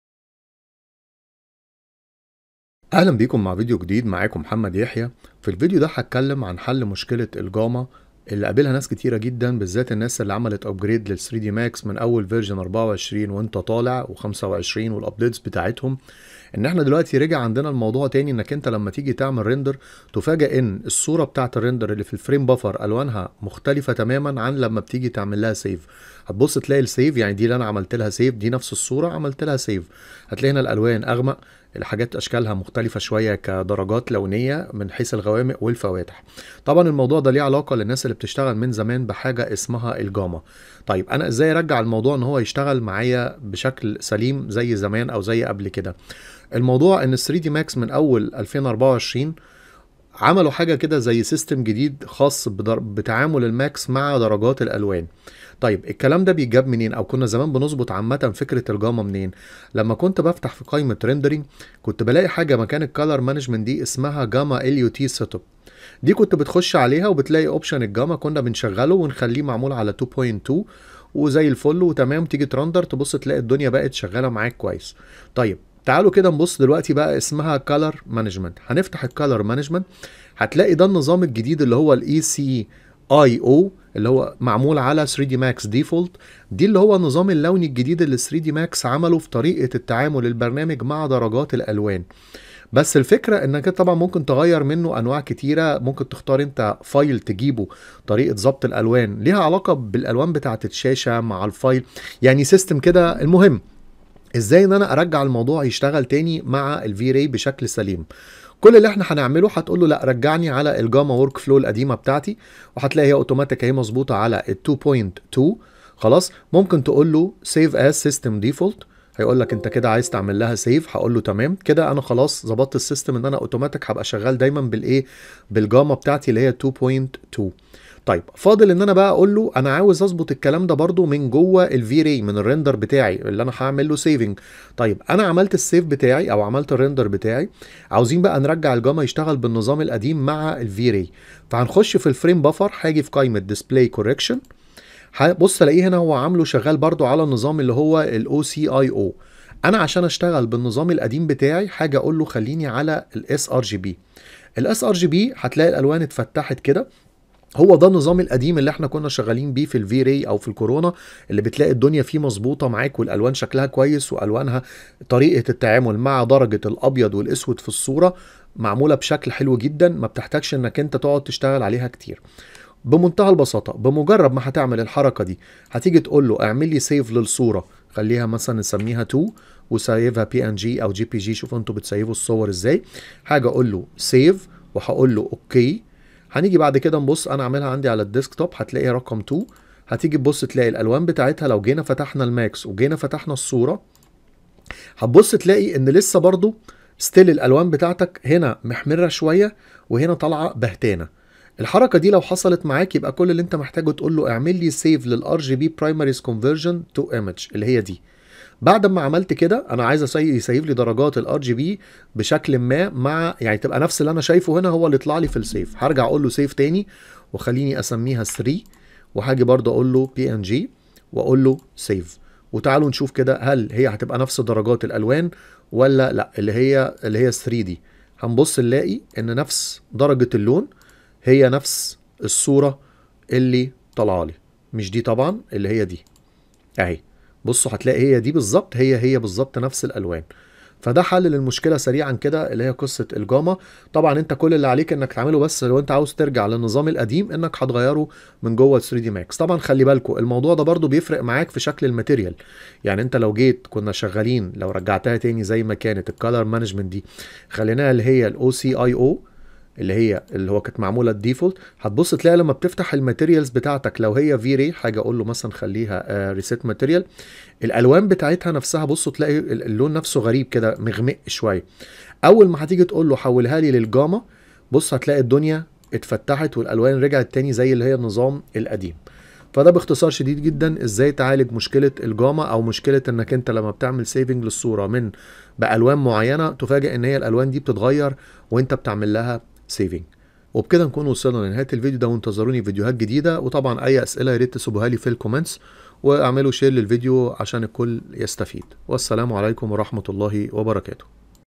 3dmax 2024 and higher Gamma fix
In this video I show you how to correct the gamma problem by exporting your render to image. With the new version of 3dsMax and V-Ray there is a gamma problem when saving your render. Here's a solution.
Gamma workflow problem
OCIO.

في هذا الفيديو، هشرح  لك كيفية تصحيح مشكلة  الجاما عن طريق تغيير النظام اللونى إلى صورة. مع حل   مشكلة جاما عند حفظ العرض
إليك الحل
مع نسخة
vray 7 render
with OCIO